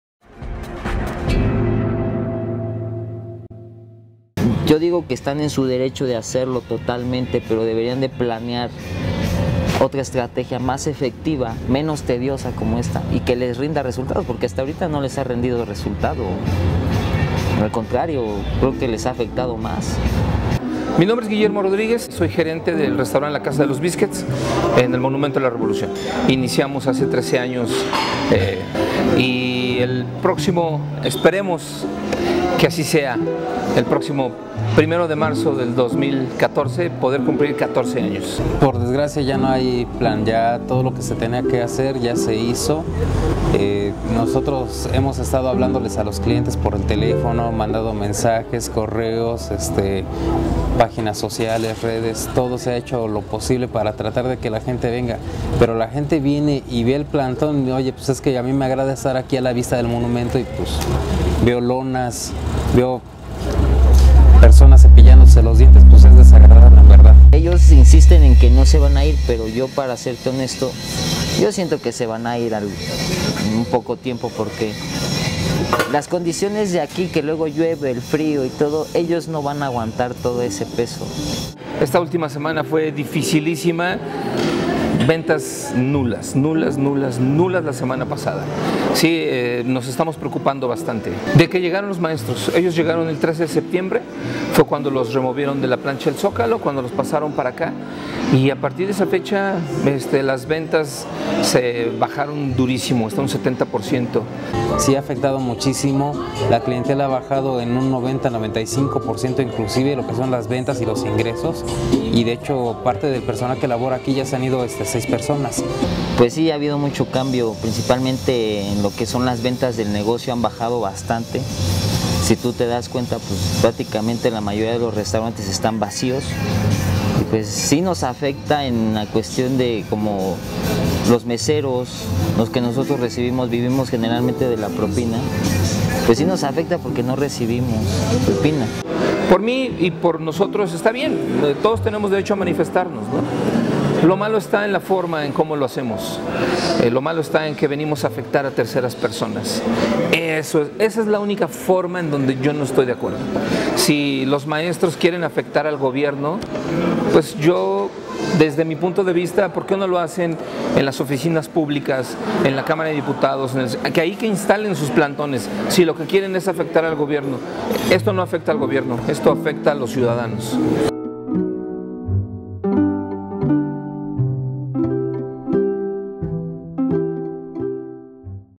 Negocios moribundos en el Monumento a la Revolución
Los restaurantes ubicados en el perímetro del Monumento a la Revolución se encuentran en una situación financiera crítica debido a la disminución de sus ingresos como resultado del plantón de la Coordinadora Nacional de Trabajadores de la Educación (CNTE).